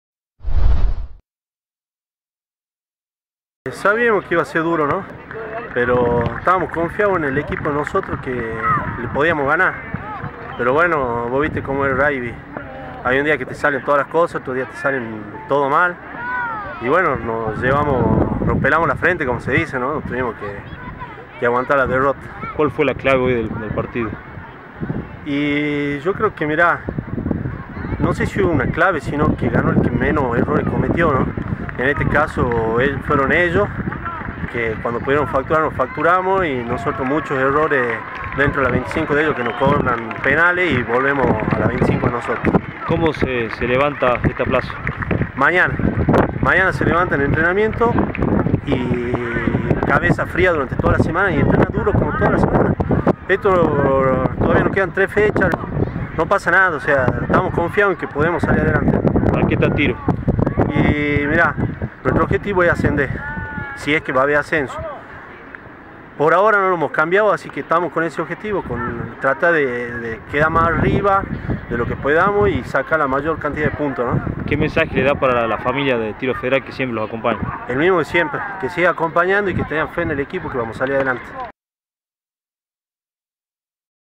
Roberto Pesci (Capitán de Tiro Federal)
Tiro Federal no tuvo una buena tarde hoy en su reducto, cayó vencido por 19-21 ante Santiago Rugby (18/09/16).